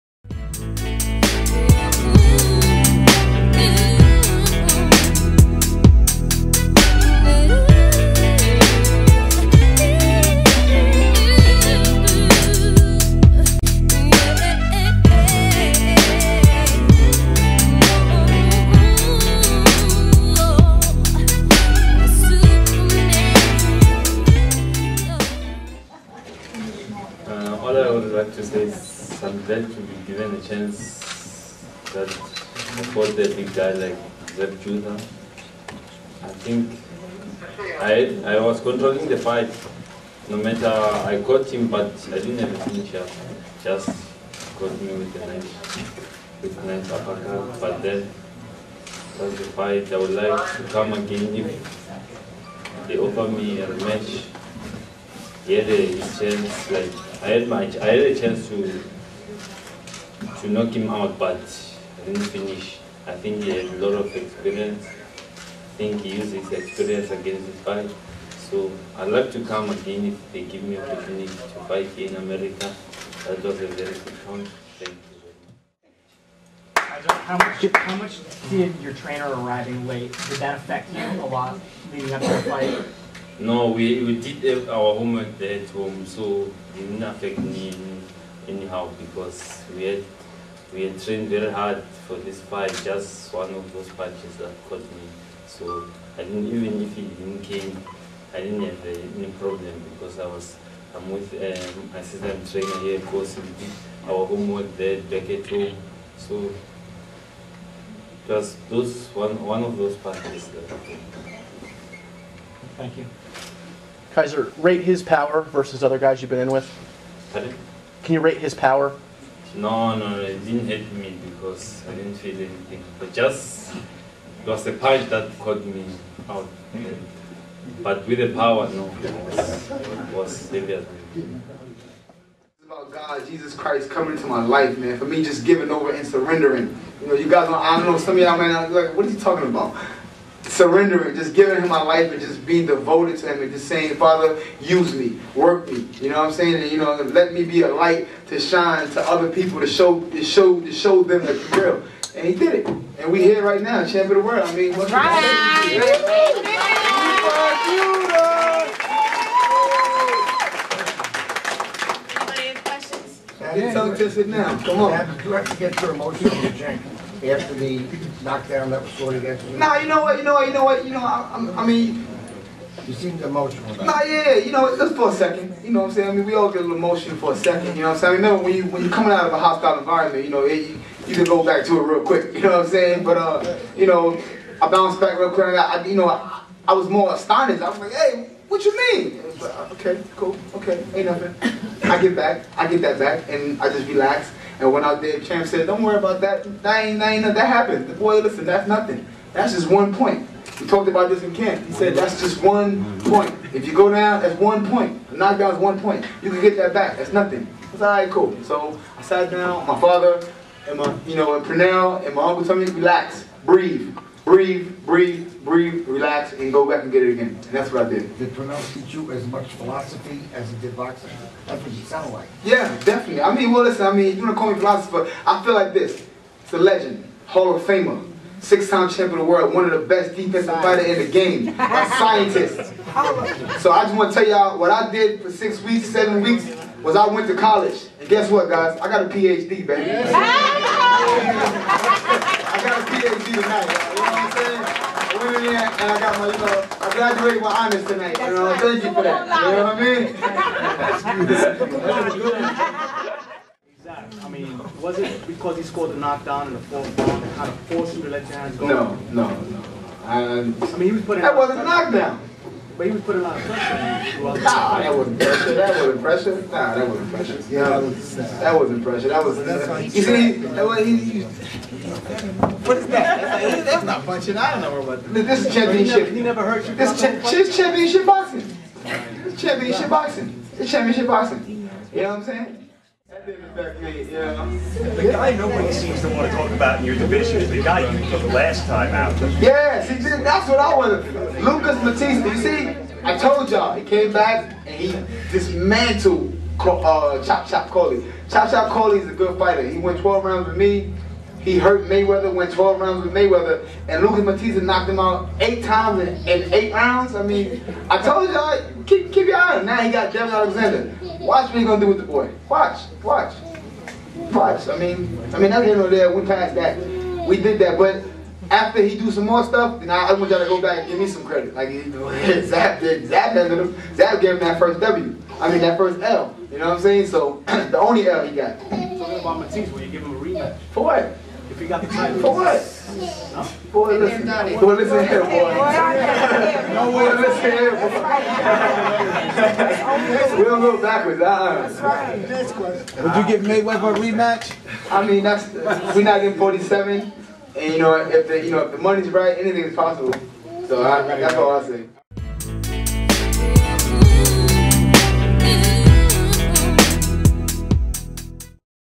Zab " Super " Judah VS Kaizer Mabuza Post Fight Press Conference Prudential Center, NJ
Zab "Super" Judah (41-6, 28 KOs) won the vacant IBF junior welterweight championship with a seventh round TKO against Kaizer Mabuza (23-8-3, 14 KOs) on 3/5/11 at the Prudential Center in Newark, New Jersey. Mike tyson was sitting ringside supporting Judah and Pernell Whitaker was in Judah's corner.
Video By Bradley orengo, Francisco Guzman.